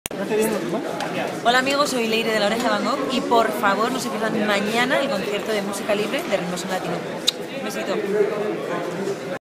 LODVG- Musica Libre
Leire Invitando al concierto de Musica Libre

Derechos : Ritmoson Latino